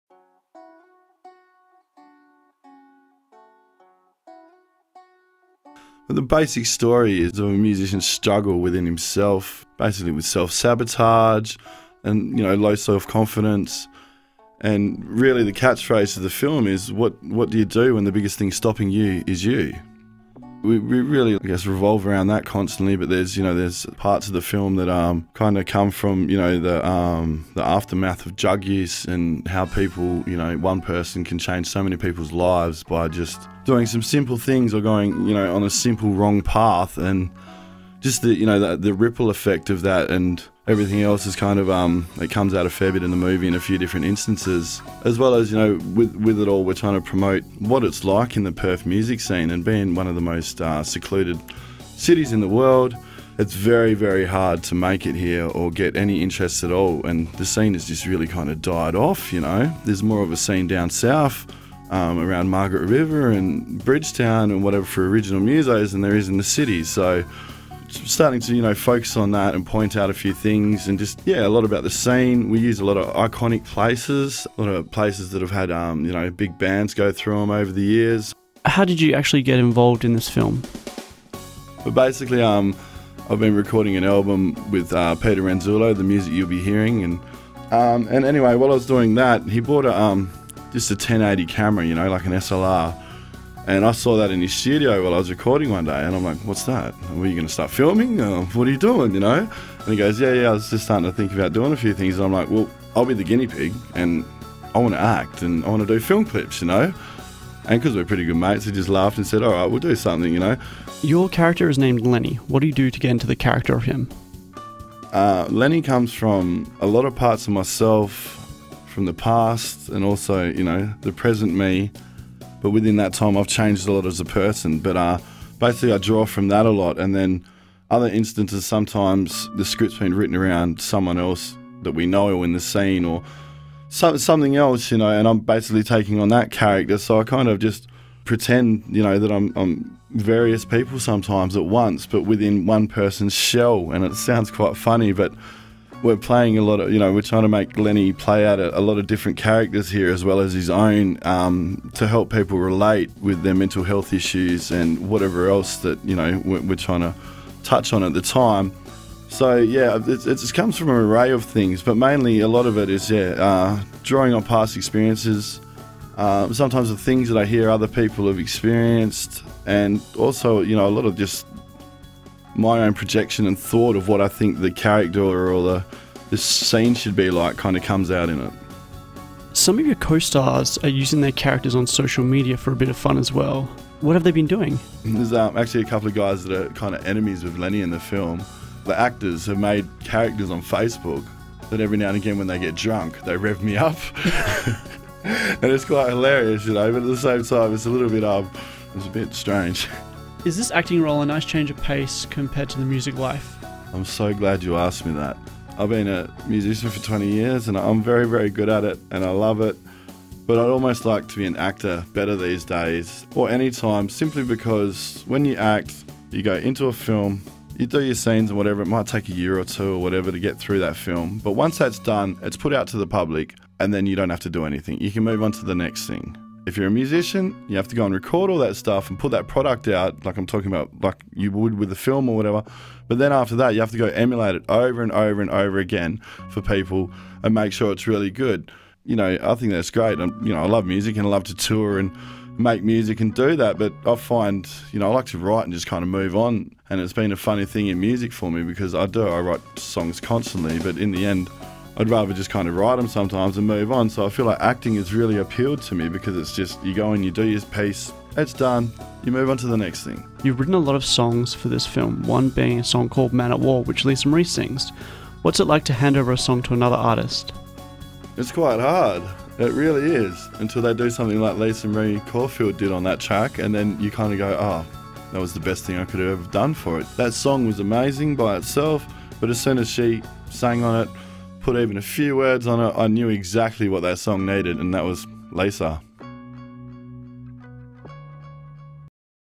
Glenn Herbert Interview - Curtin FM
This is an interview conducted by Curtin FM with Anticipation's main cast member Glenn Herbert, regarding his role and some back story to the film itself.